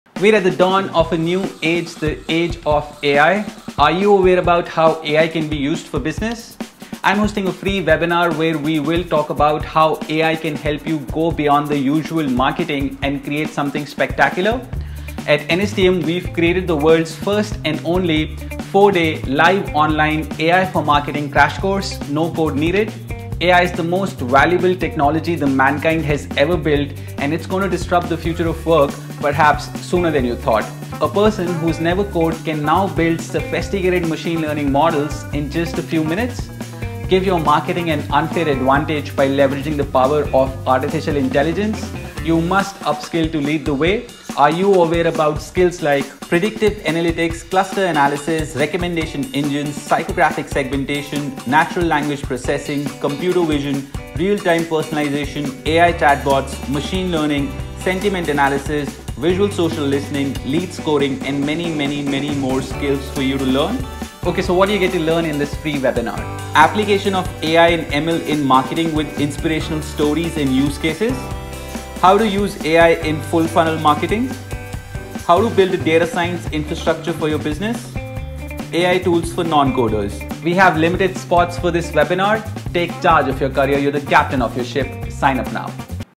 Free A.I. For Marketing Webinar - Limited Spots - Sign Up Now
Attend the free  A.I. for marketing webinar. Master all the concepts and terms of artificial intelligence and machine learning. Understand how A.I. can be practically applied in business –predictive analytics, clustering, recommendation engines, natural language processing, computer vision and A.I. automation chatbots.